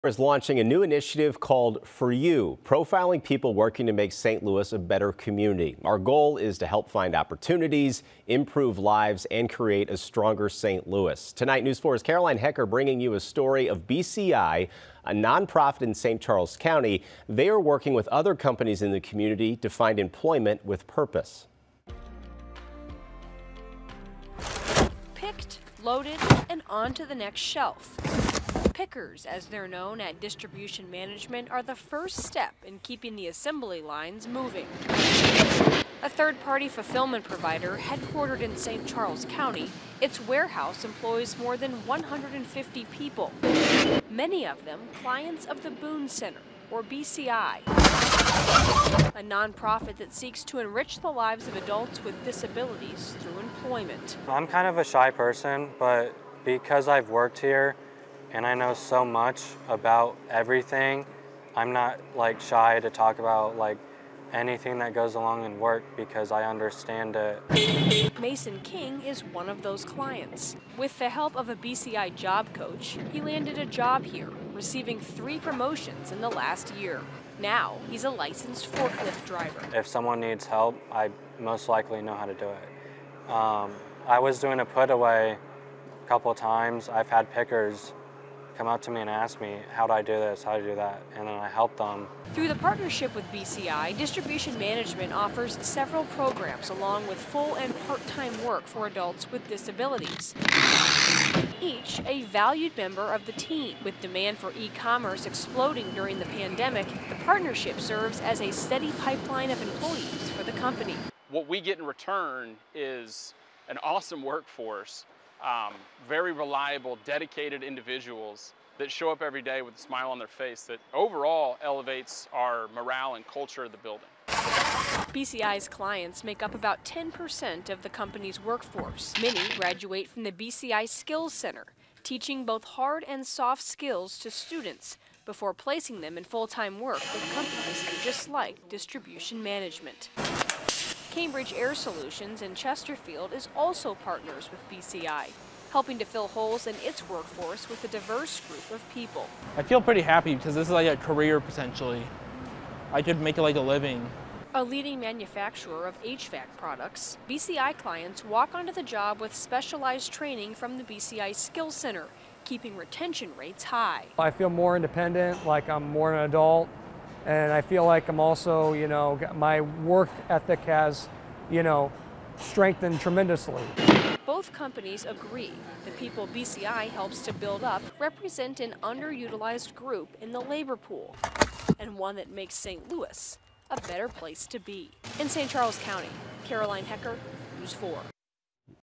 4 You: St. Charles County non-profit helps adults with disabilities achieve independence
Boone Center Inc., or BCI, is a non-profit that seeks to enrich the lives of adults with disabilities and their families.